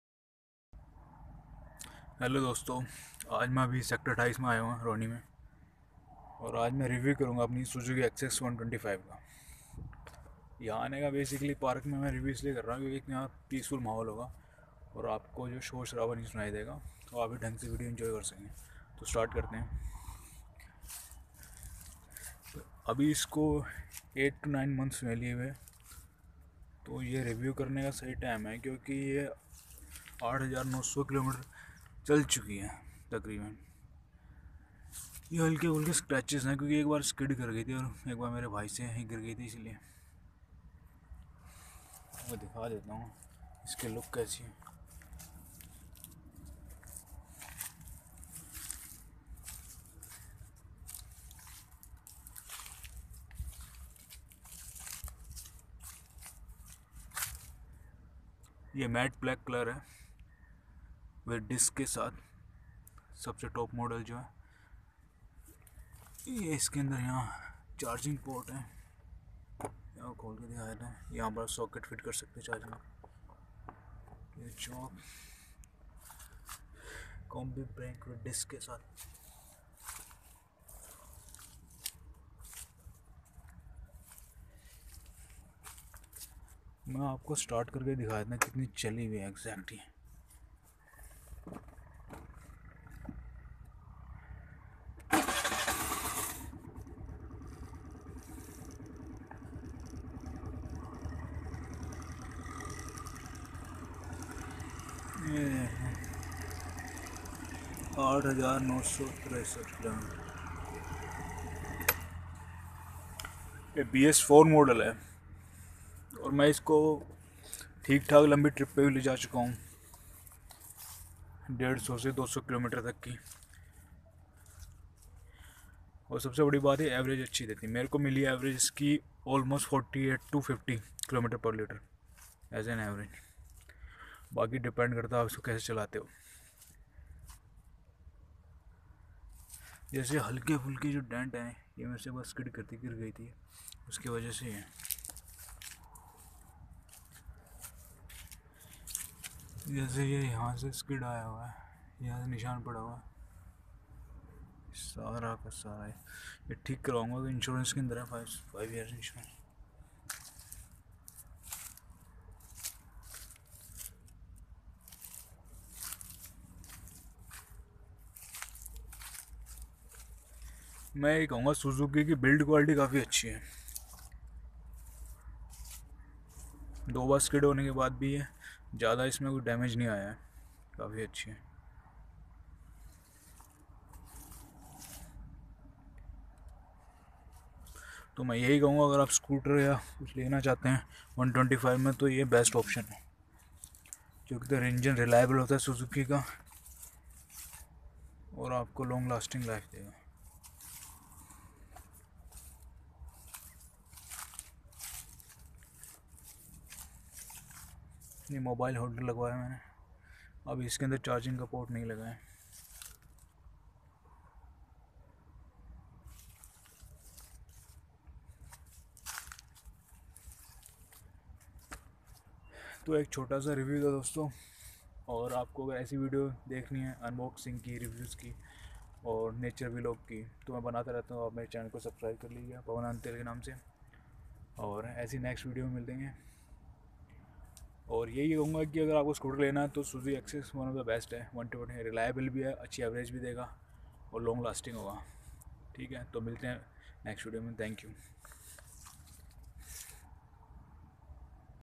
My Suzuki access 125 review disc variant bs4(after 9800 km)
This is the  first review of my Suzuki access 125 mat black which i bought in April 2019 it run almost 9800 km in 9 months very reliable so enjoy the video and please subscribe my channel and share my videos peace 😊